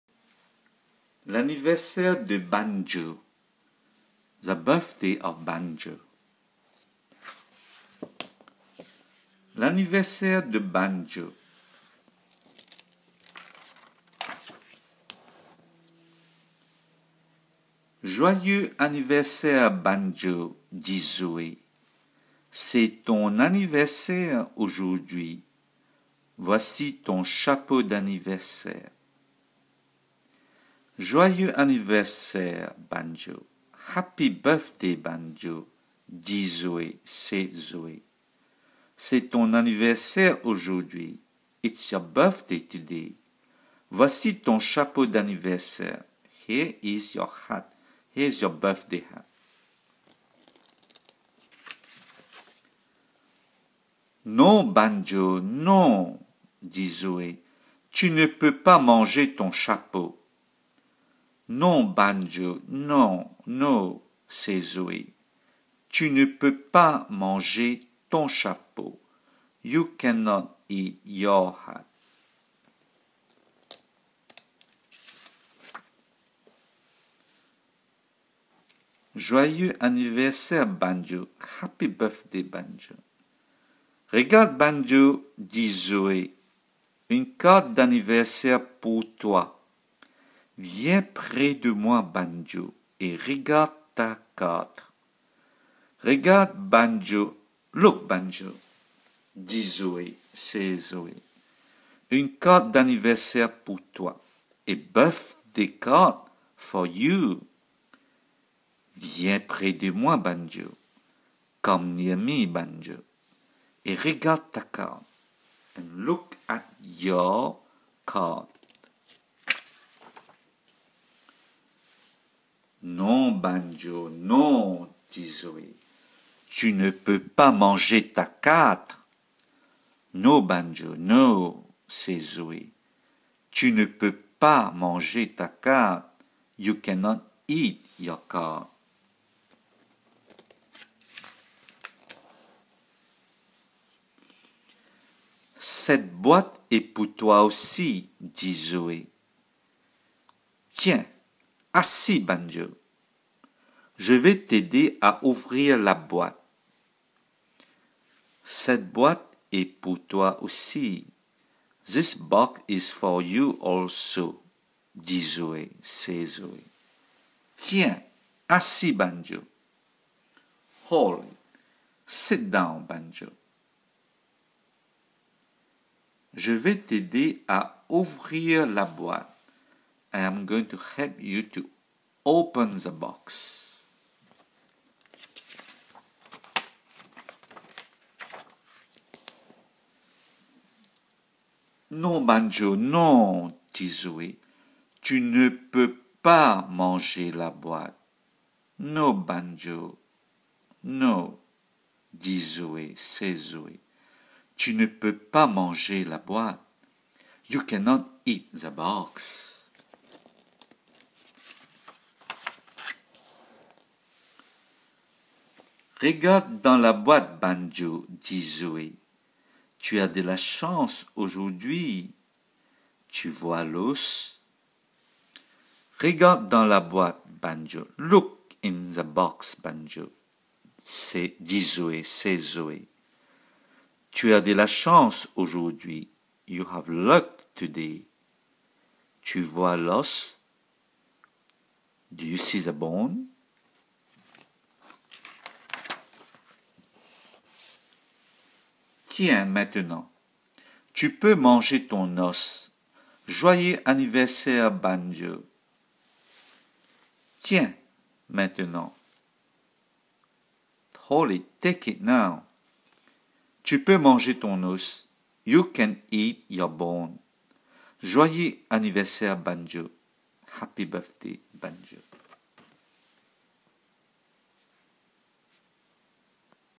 L'anniversaire de Banjo - GB+ 7
Histoire par Annette Smith - Illustrations par Pat Reynolds  - Narration par Kampabear
Apprendre à lire - Learn to read French books
Livres GB Plus - Niveau 7